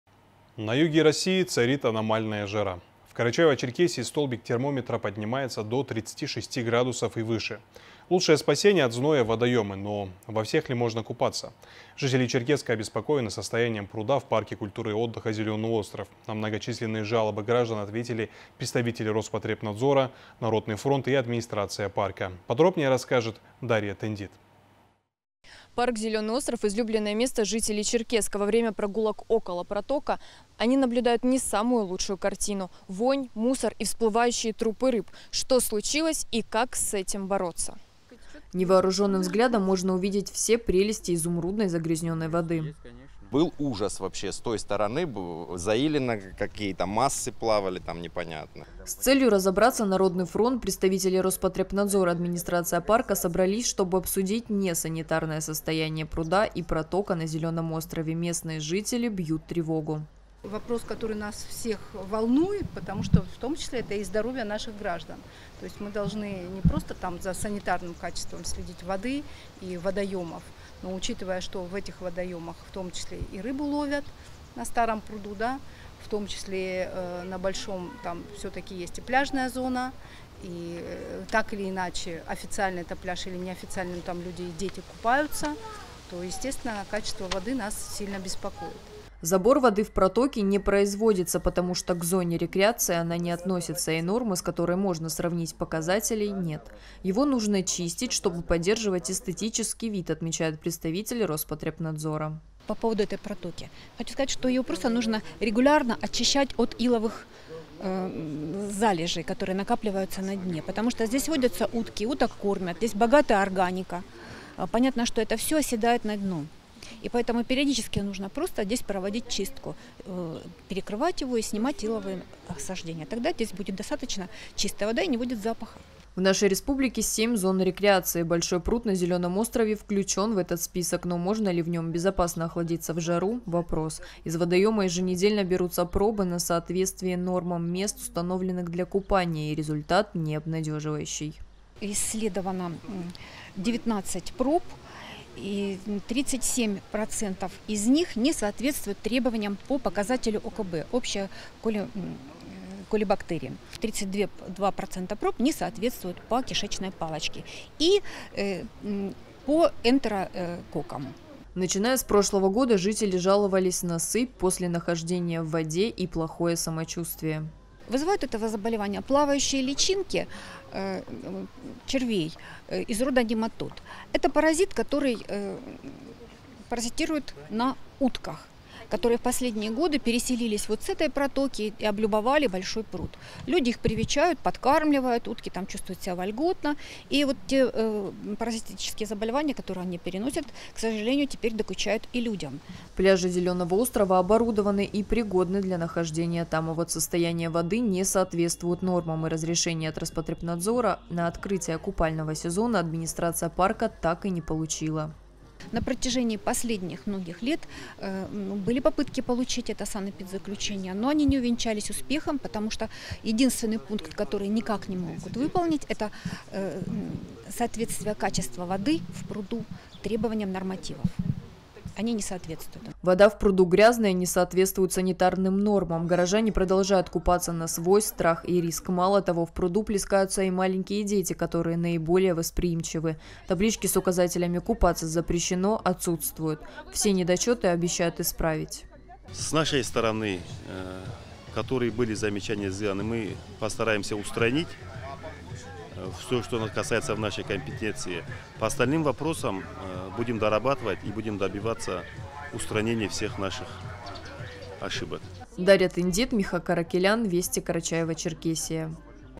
Жители Черкесска обеспокоены состоянием пруда в парке культуры и отдыха "Зеленый остров"
На юге России царит аномальная жара. В Карачаево-Черкесии столбик термометра поднимается до 36° и выше. Лучшее спасение от зноя водоемы, но во всех ли можно купаться? Жители Черкесска обеспокоены состоянием пруда в парке культуры и отдыха "Зеленый остров". На многочисленные жалобы граждан ответили представители Роспотребнадзора, Народного фронта и администрации парка.